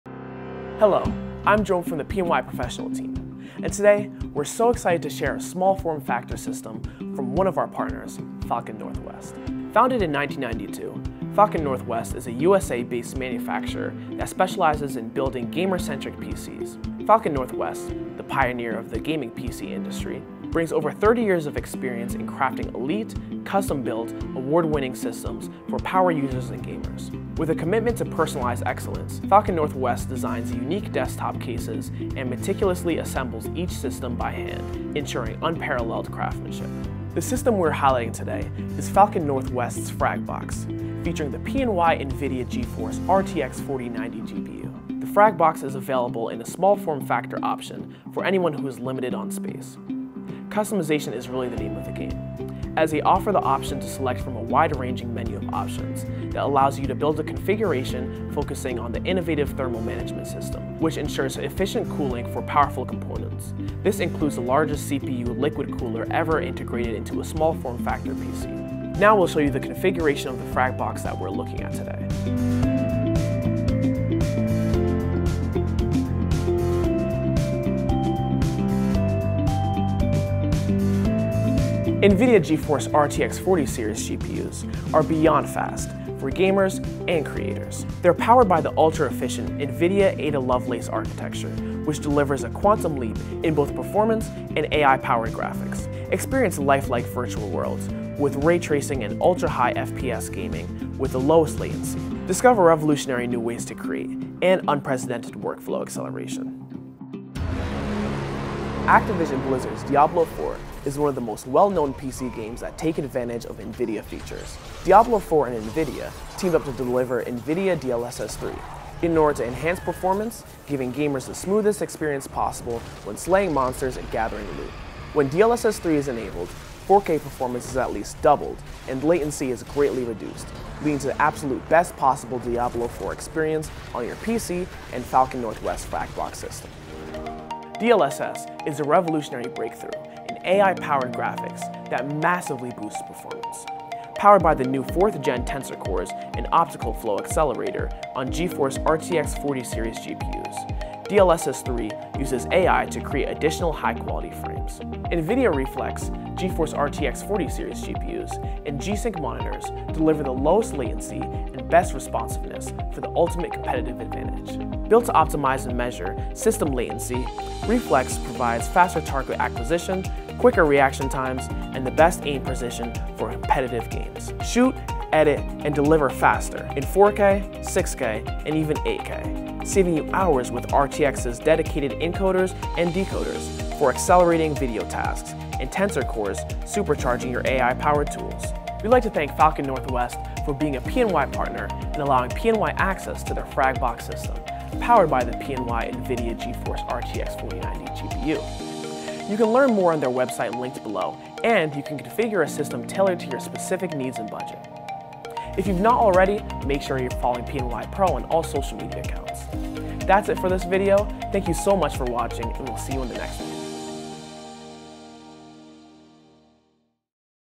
Partner Spotlight - Falcon Northwest | Fragbox with PNY NVIDIA GeForce RTX 4090
PNY would like to thank @Falcon_Northwest for being a partner and allowing us access to their FragBox system powered by the PNY @NVIDIAGeForce RTX 4090 GPU. You can learn more about the Fragbox and other options Falcon Northwest offers by visiting their website and configuring a
system tailored to your needs.

If you would like to learn more about PNY Products, please fill out this form: